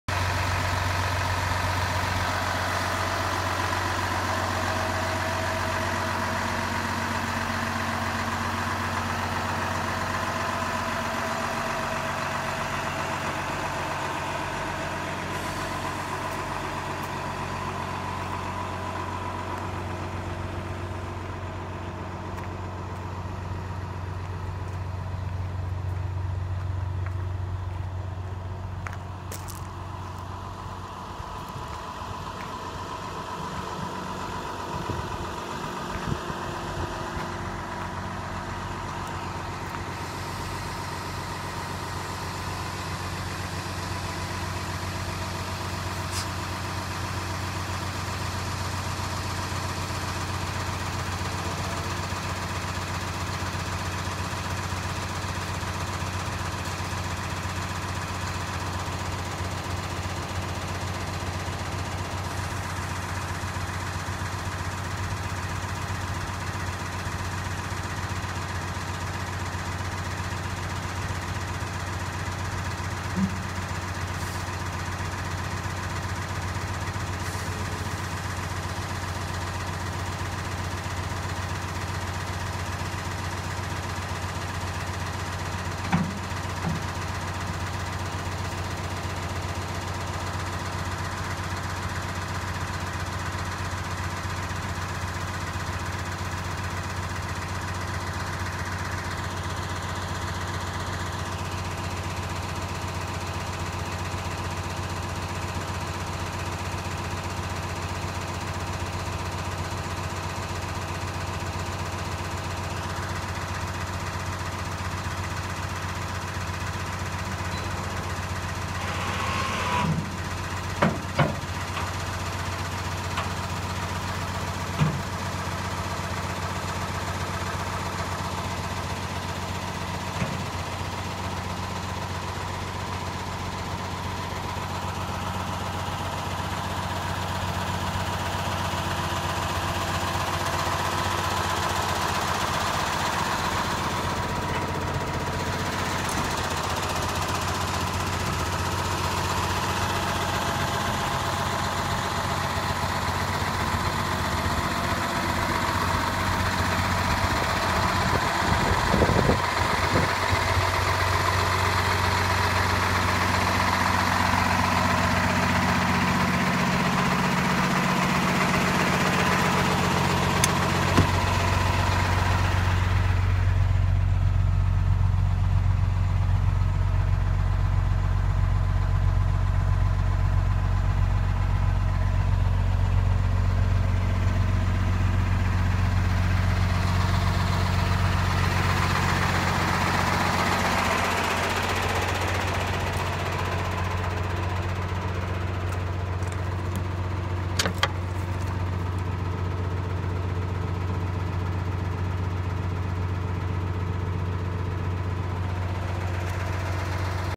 For Sale - 2014 Kenworth T-370 Retriever Body Equipment Hauler
for pricing and availability. For Sale - 2014 Kenworth T-370 Retriever Body Equipment Hauler, Paccar PX-9 350 HP, Allison 3000 Series Auto, A/C, PW, 56000 lbs Gvw, FA 16, RA 40, Air Ride Susp, Engine Brake, Aluminum Tool boxes, 26'-0-" Retriever Body with 4'-0" folding Deck could have 30'-0" of Deck plus folding Pneumatic folding ramps, Adjustable Electric Winch with Remote Control ,CLEAN UNIT